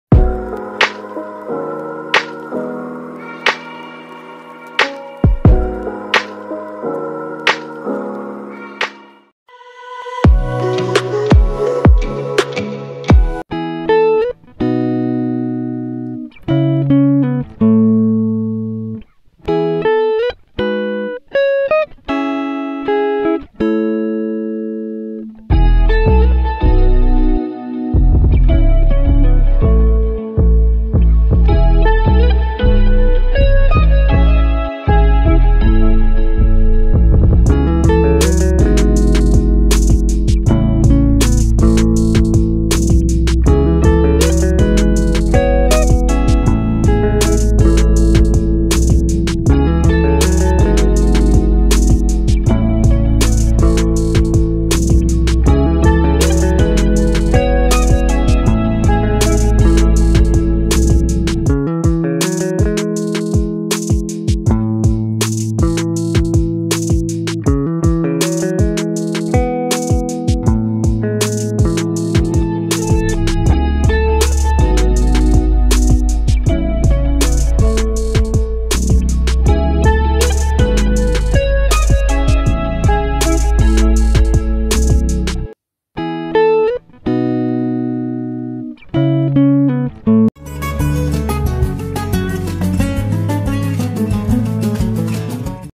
WHITENING MASK | YOUR SKIN WILL TURN WHITE, SMOOTH |HOMEMADE FACE MASK
homemade face mask super nice because you see your skin whitens and smooth, clean